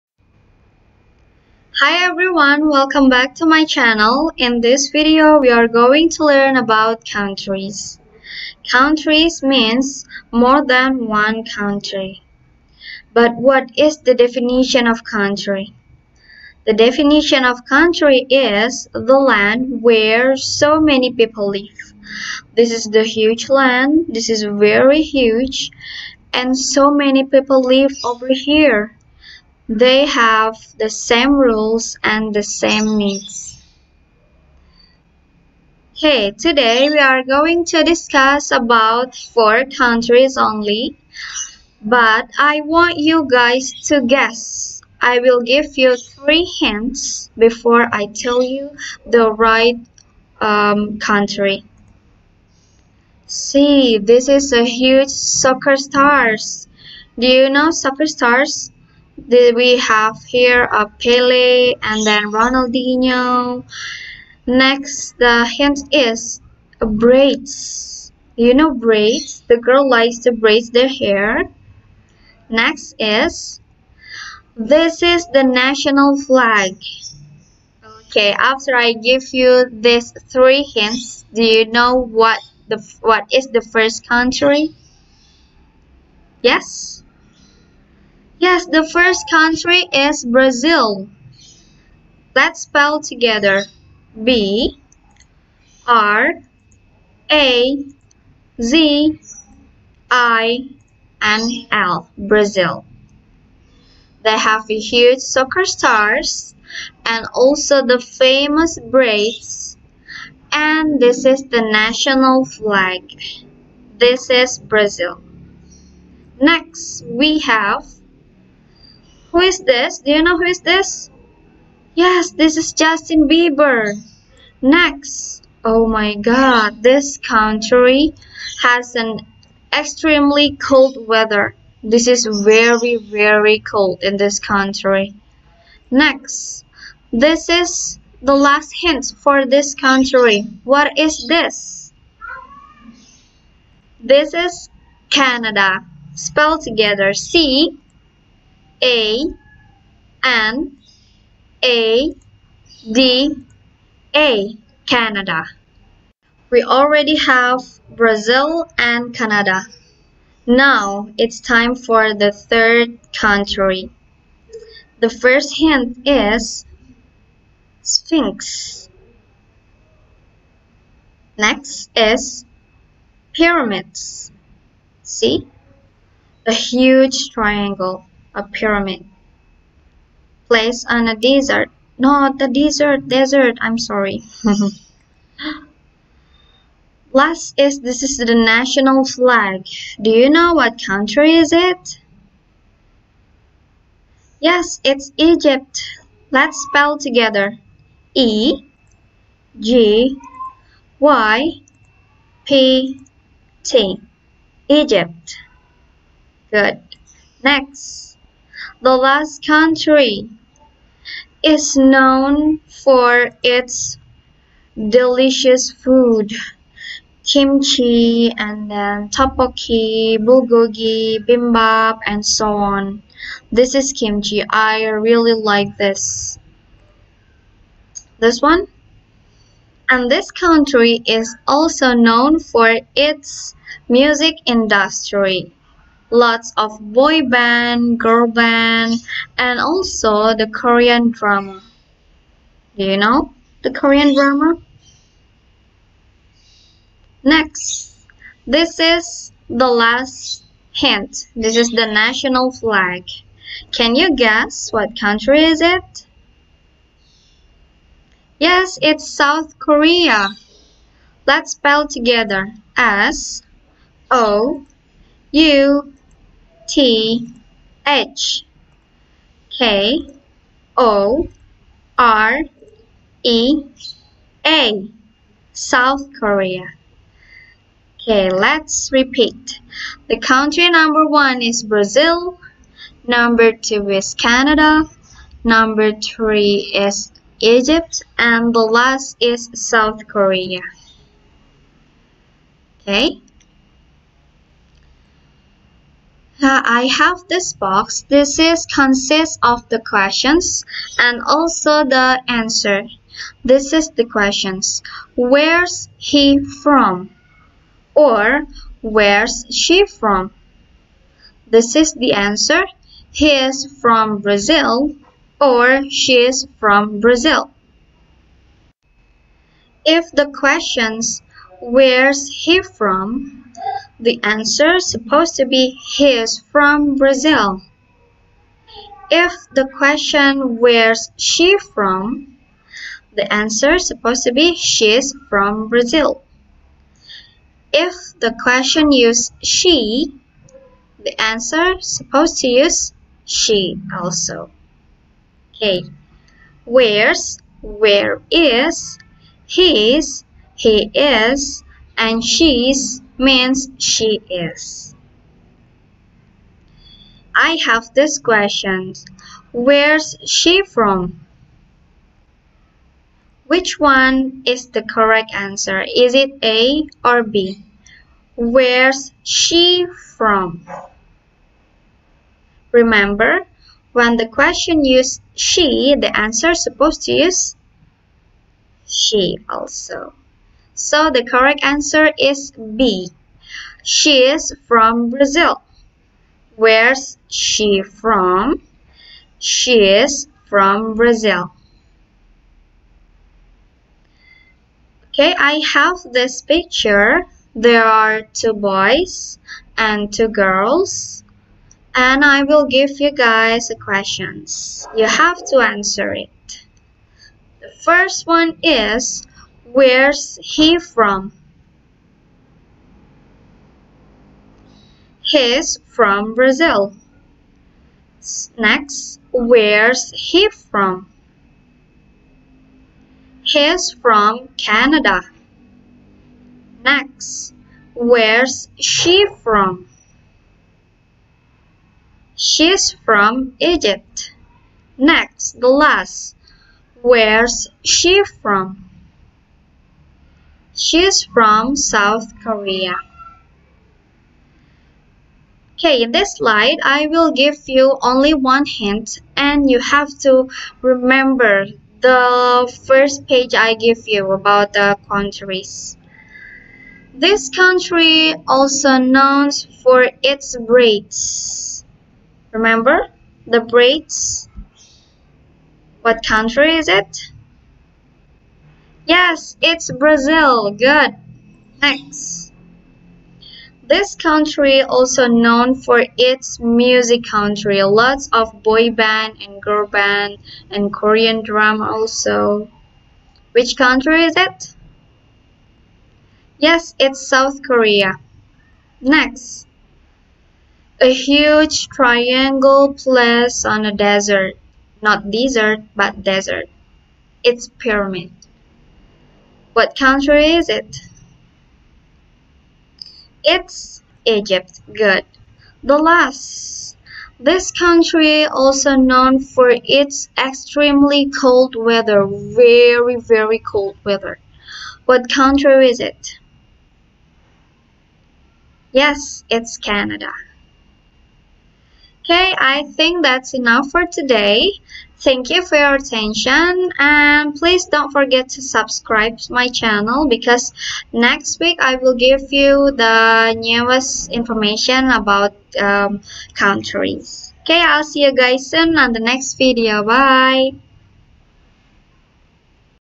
Countries around the world 2nd grader
Learn from Every body up 2 english book page 72-73. have fun kiddos!